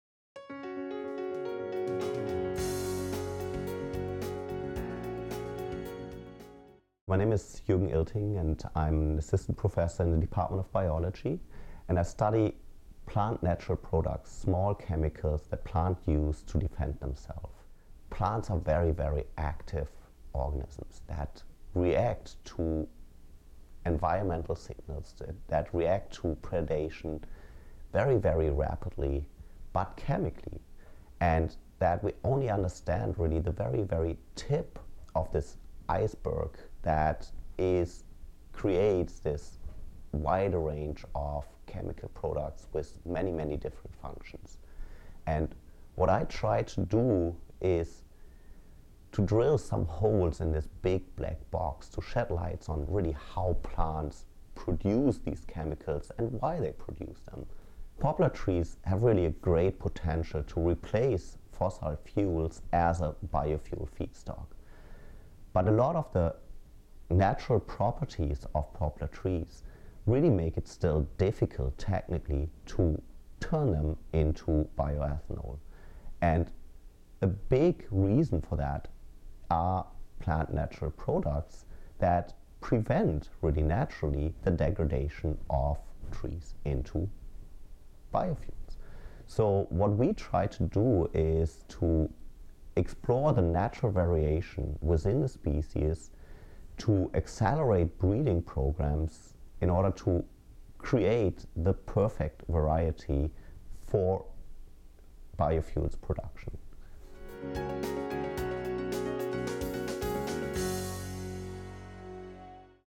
Faces of UVic Research: Juergen Ehlting
Dr. Juergen Ehlting is a professor with the Department of Biology at the University of Victoria Faculty of Science. Hi is employing the tools of molecular genetics and genomics to understand the biosynthetic routes of plant natural products, especially in forest trees. In this video, he discusses these plant natural products and their applications in society.